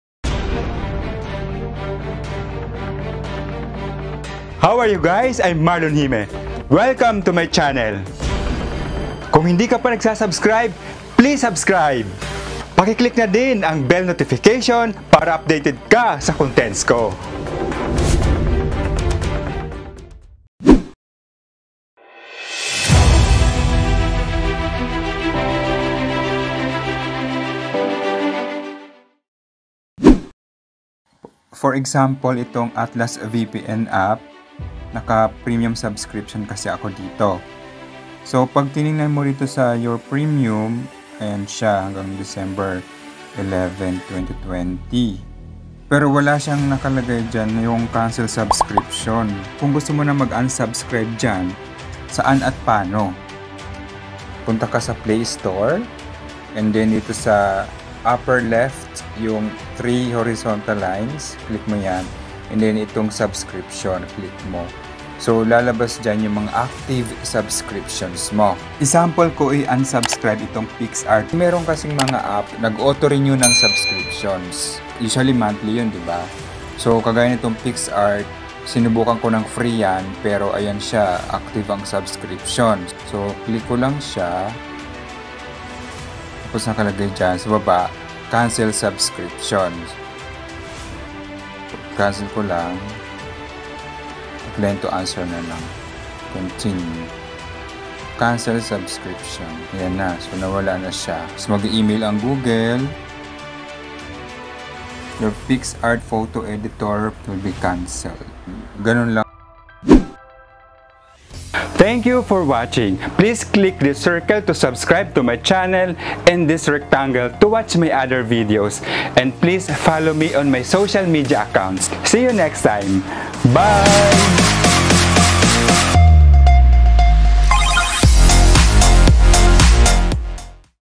HOW TO CANCEL APP SUBSCRIPTION in Google Play, Play Store & Android MUST WATCH!!! (2020)
In this video, I will share with you the way to cancel your App subscription in Google Play, Play Store & Android.

Some of the App don't have the option to cancel the subscription in the App itself, so the best way is to cancel it in the Google Play or Play Store.

Deleting only the app will not cancel the subscription.

HOW TO PAUSE PAYMENT OR CANCEL APP SUBSCRIPTION (2022)｜Google Play, Play Store and Android